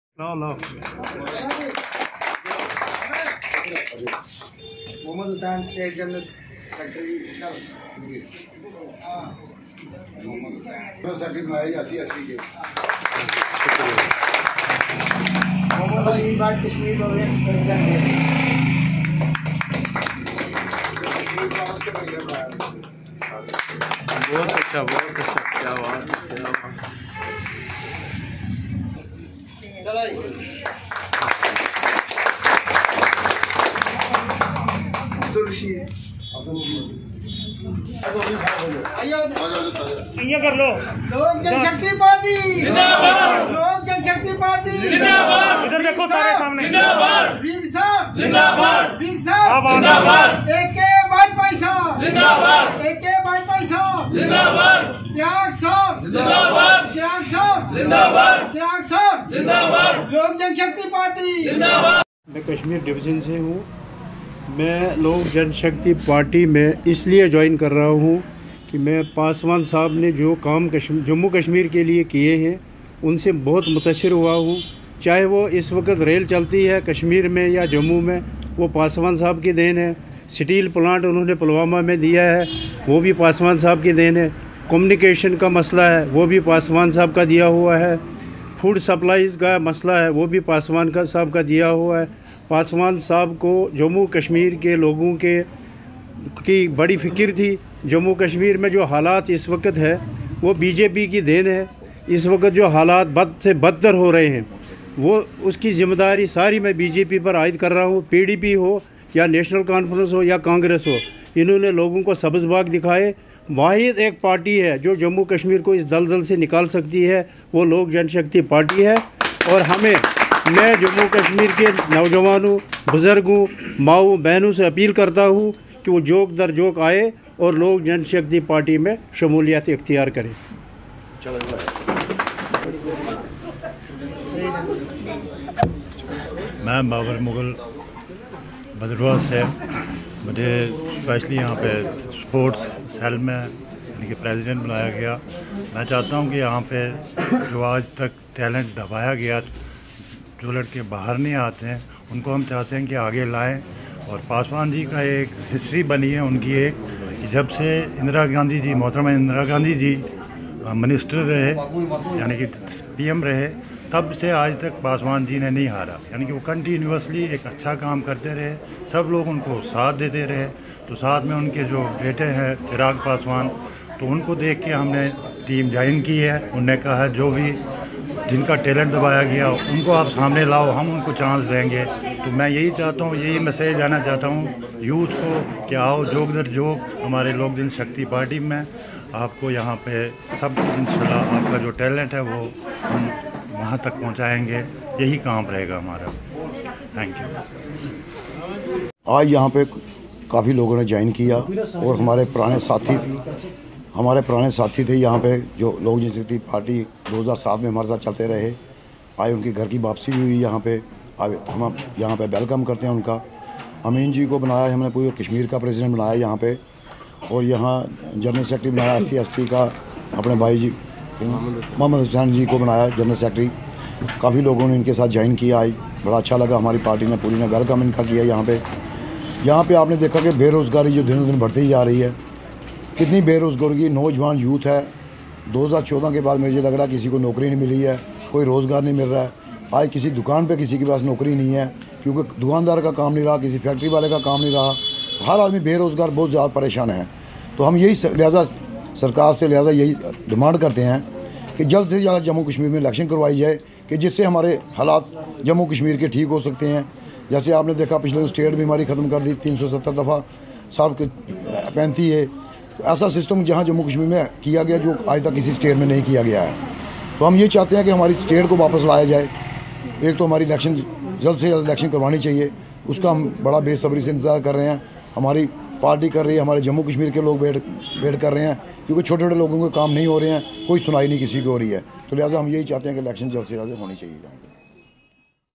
J&K YOUTHS JOIN LOK JANSHAKTI PARTY IN JAMMU II RAFIQ MALIK DEMAND EARLY ELECTION IN J&K I BOLD NEWS
Several youths join the Lok Janshakti Party in Jammu and on this occasion, the president of J&K Mr. Rafiq Malik demanded an early election in the Jammu and Kashmir in the interest of the public.

Video Journalist: Ashok Anand